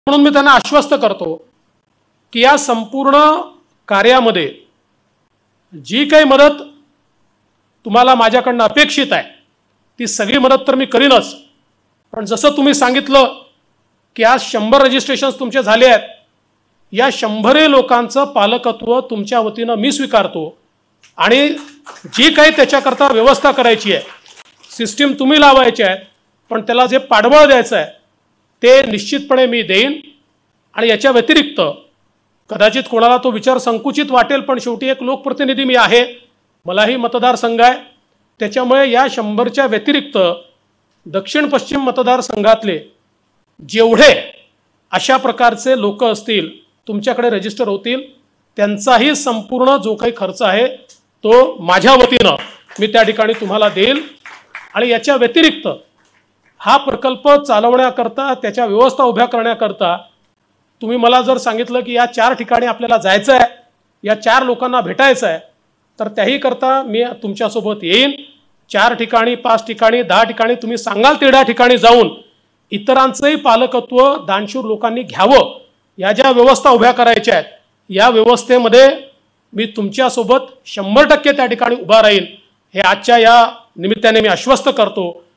श्री देवेंद्र फडणवीस यांनी स्वीकारले 'सोबत' पालकत्व प्रकल्पात नोंदणी झालेल्या 100 लोकांचे पालकत्व!
या संस्थेकडे आलेल्या पहिल्या 100 बालकांच्या नोंदणीचे पालकत्त्व मी स्वीकारत आहे. याशिवाय, माझ्या दक्षिण-पश्चिम नागपूर विधानसभा मतदारसंघातून जी नोंदणी होईल, त्यांचीही संपूर्ण जबाबदारी मी स्वीकारतो आहे.